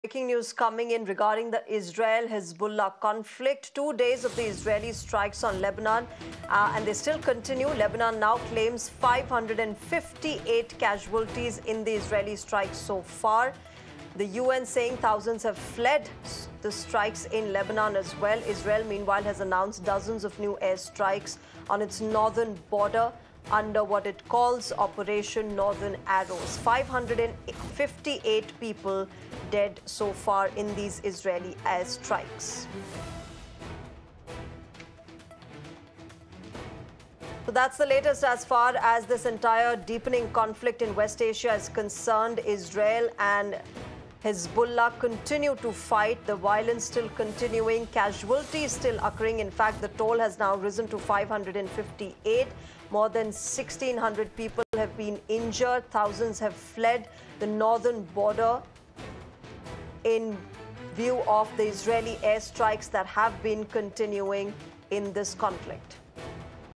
Breaking News | Israel-Hezbollah Conflict; Lebanon Claims 558 Deaths, Thousands Fled Israeli Strikes
Breaking News | Israel-Hezbollah Conflict: Lebanon claims 558 deaths, with thousands fleeing Israeli strikes.